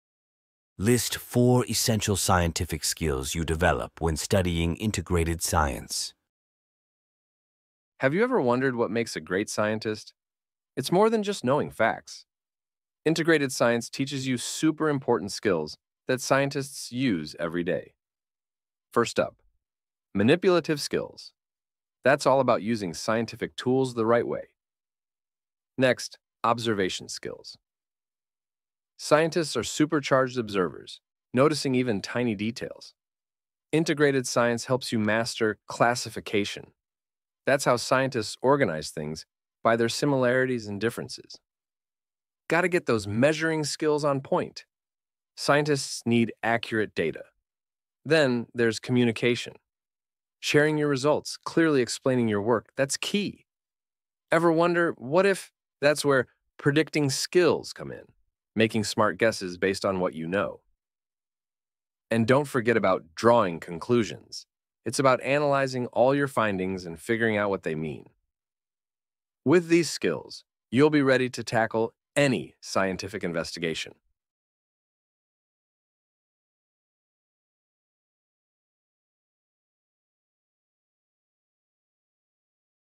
4. Grade 8 CBC Science Practice Exam #1
Are you ready to think like a scientist? Discover the awesome skills you build in integrated science.  We'll cover everything from using lab tools to making predictions. Get ready to level up your science game!